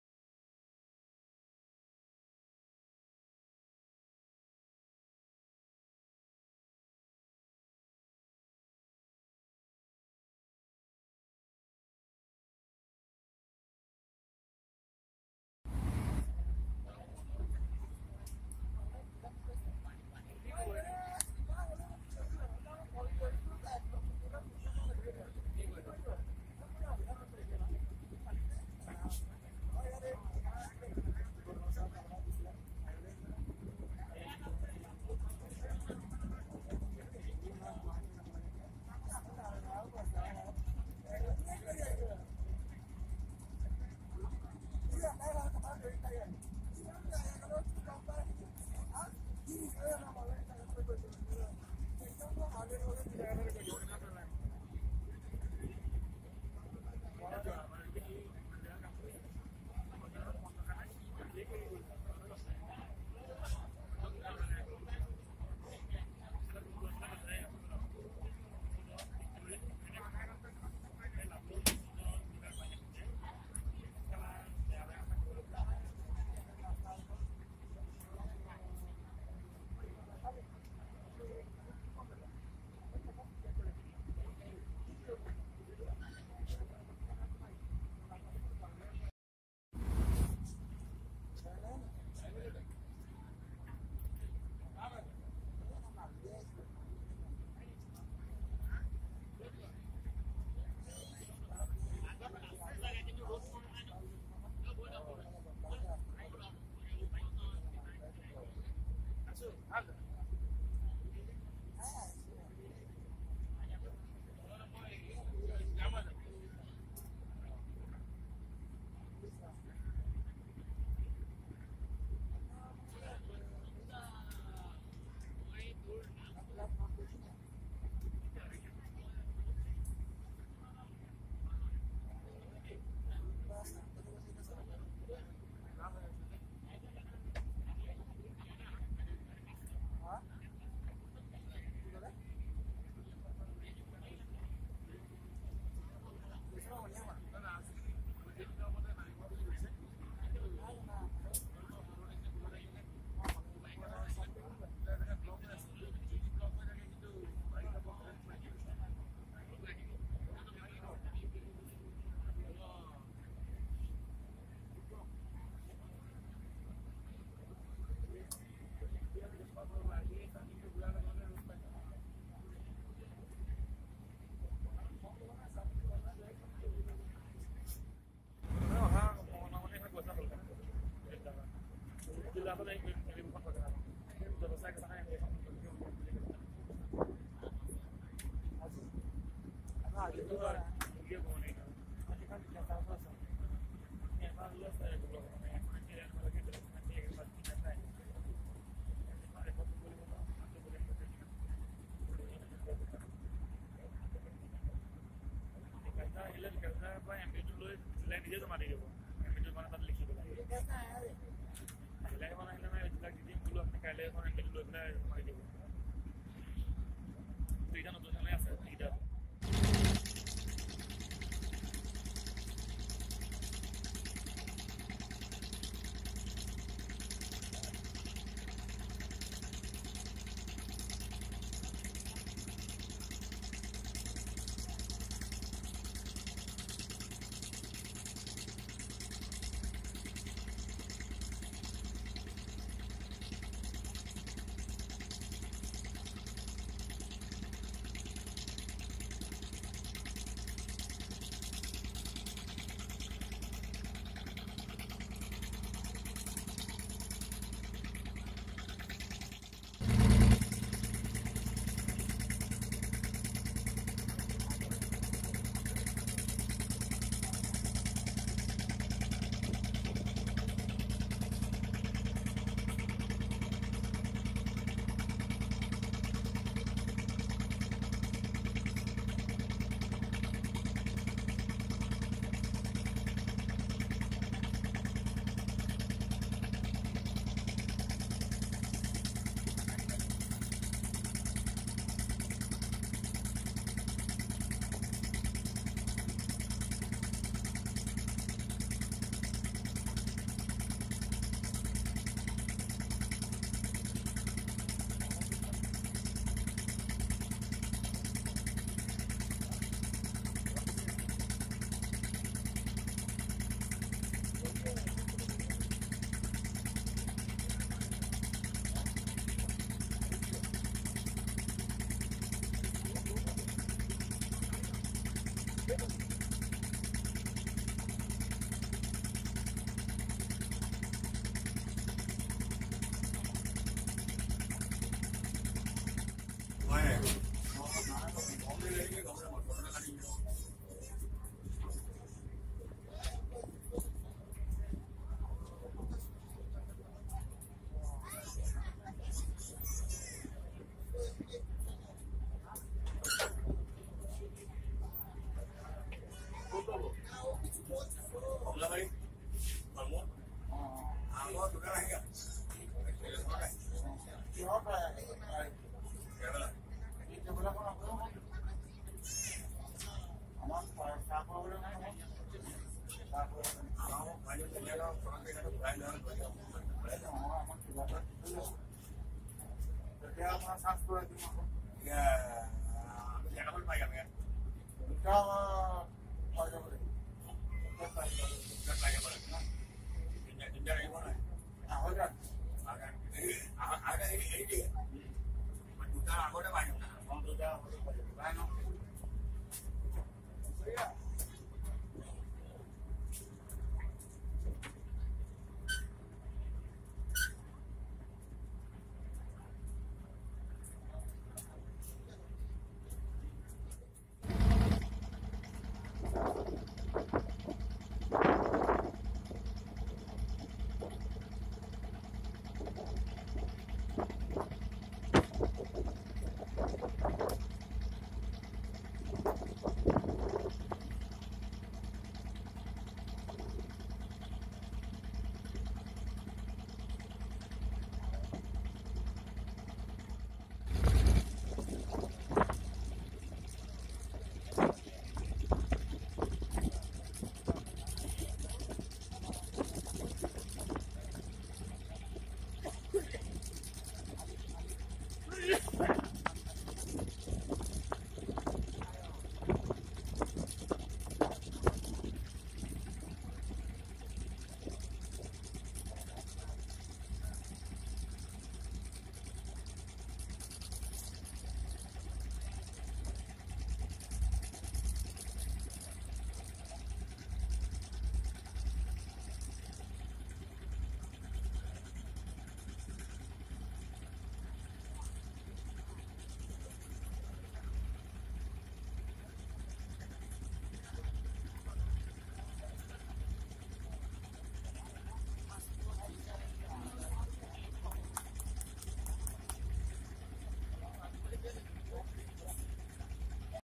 To Majuli - Largest River Island on Earth
Ferry crossing to Majuli on Brahmaputra - Largest River Island on Earth